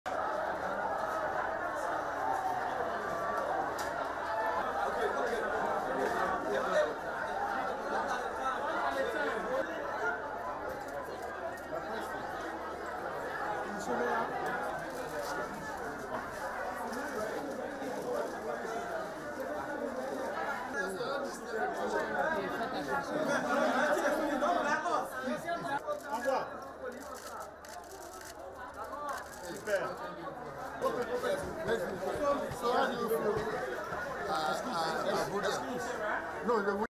Liberia: George Weah casts his ballot in presidential elections
Liberian presidential candidate and former international footballer George Weah casts his vote in the run-off presidential election. IMAGES